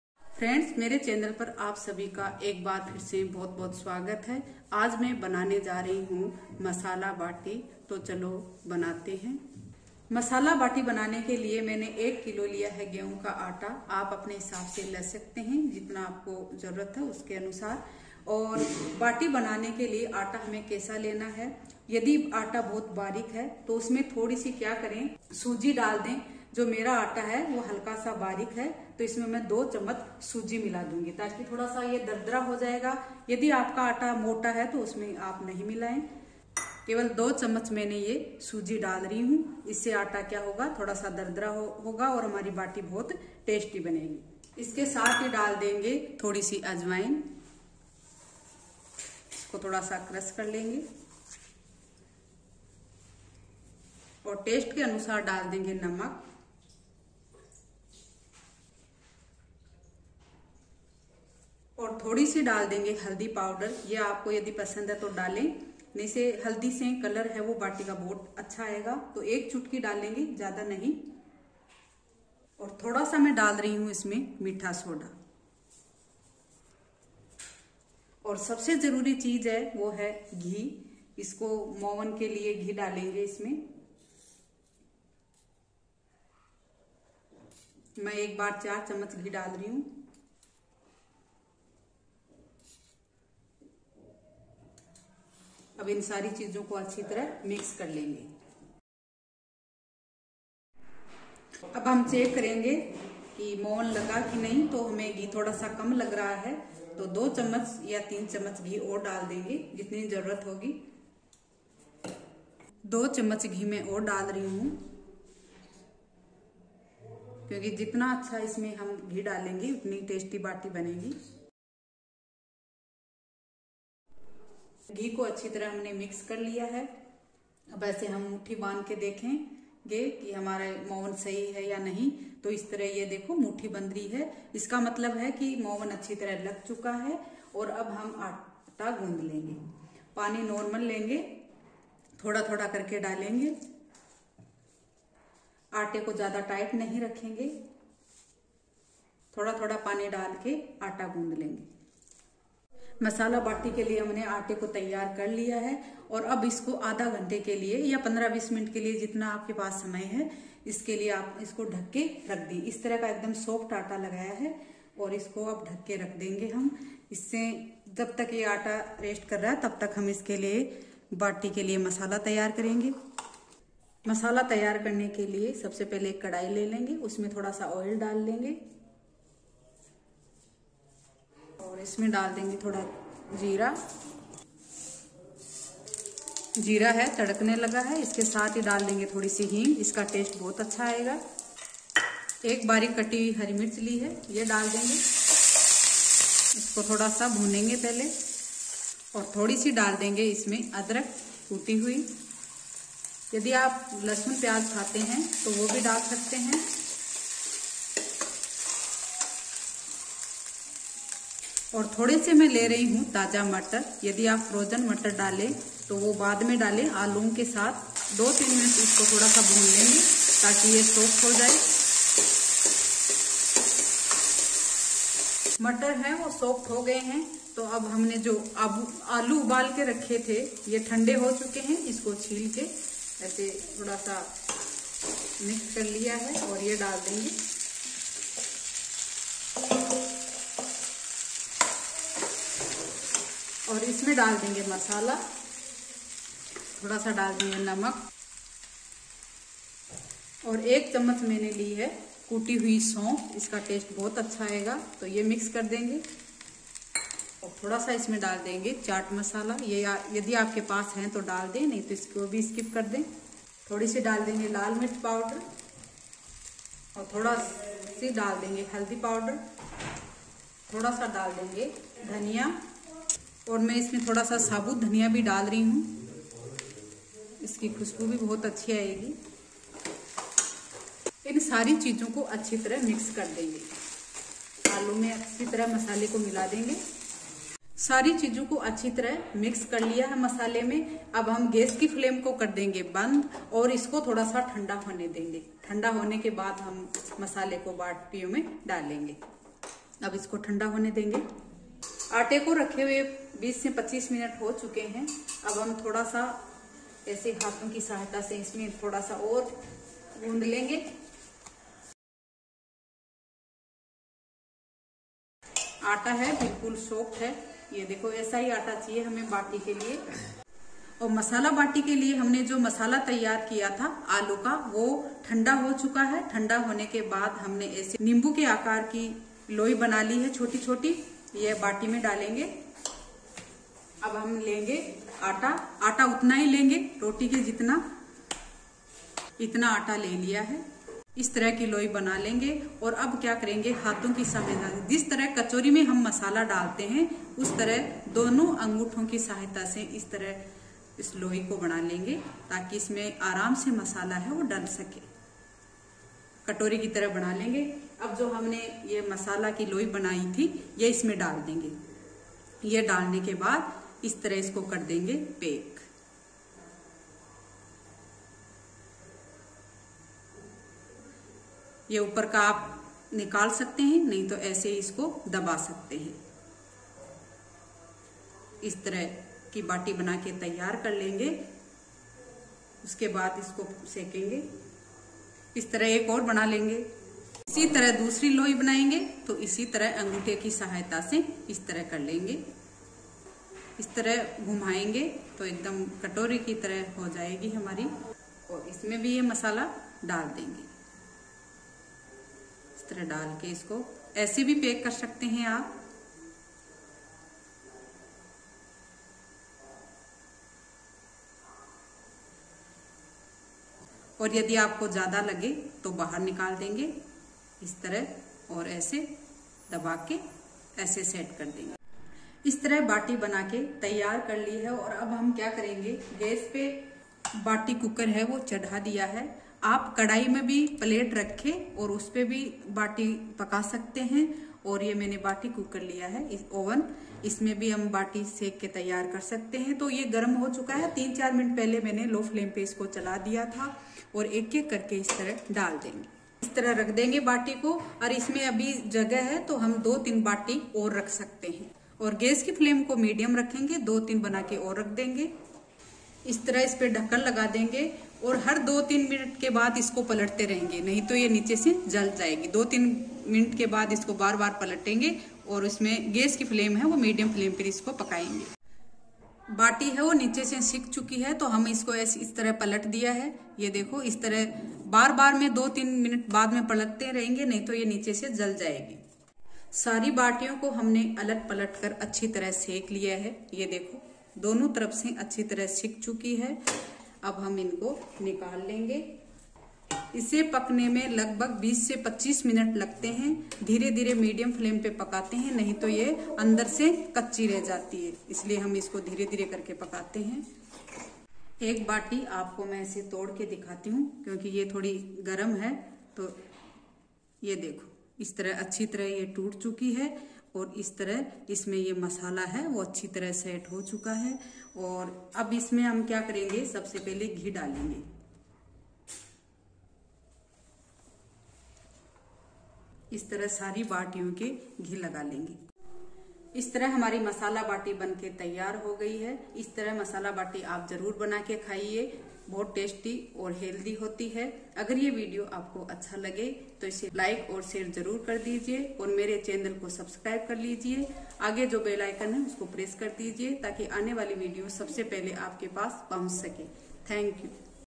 समोसा कचोरी भुलादे ऐसा गेहूं के आटे का कम तेलमें नया नास्ता | less oil snacks | Masala baati recipe
समोसा कचोरी भुलादे ऐसा गेहूं के आटे का कम तेलमें नया नास्ता | less oil snacks | Masala baati recipe@bittukirasoi